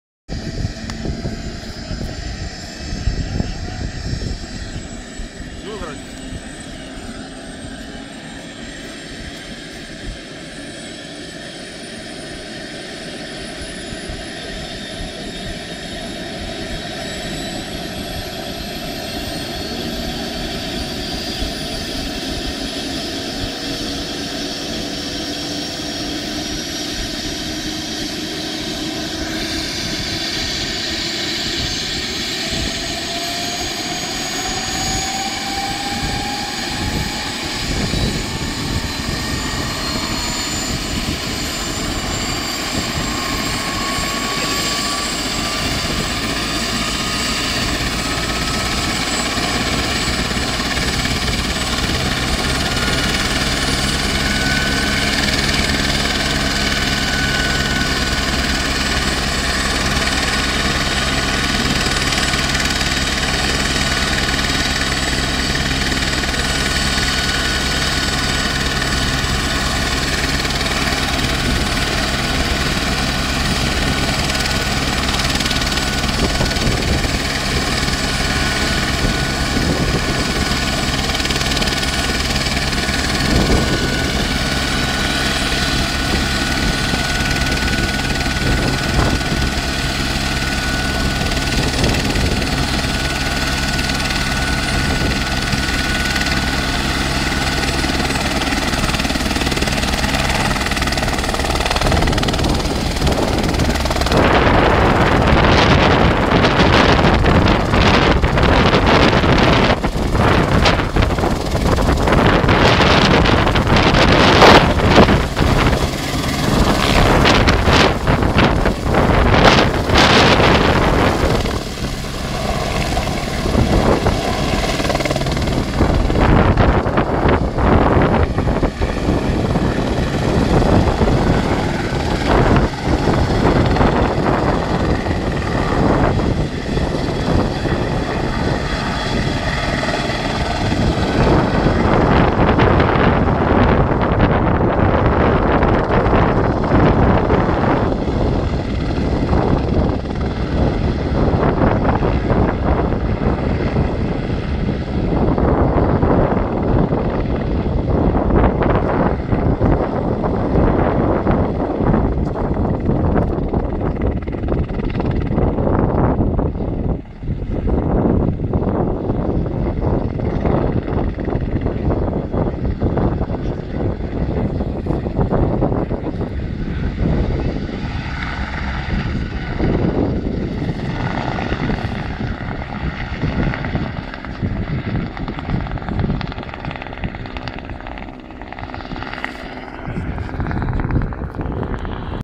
Decolagem Pegasús do Aeroporto de Montes Claros
Decolagem do Helicoptero da PMMG pegasús 3º Copaer